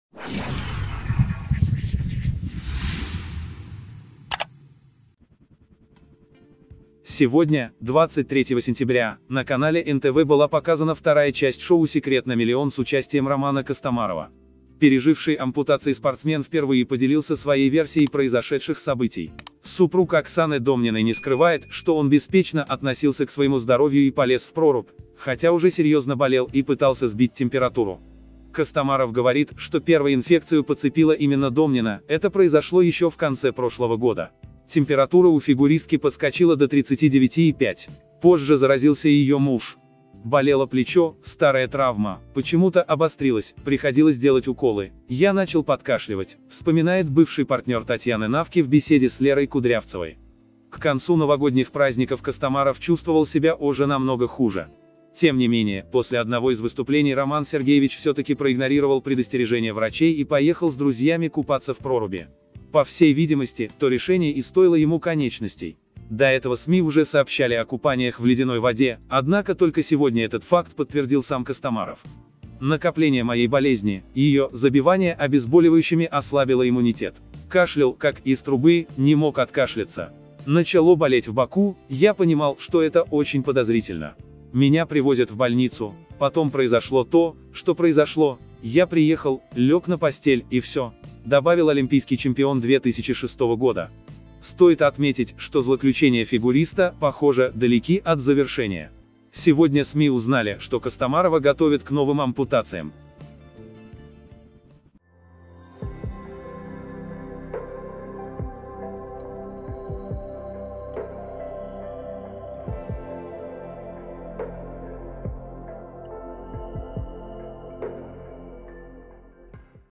Роман Костомаров признался, что сам довел себя до ампутации
Роман Костомаров признал, что купался в проруби и сам довел себя до ампутации.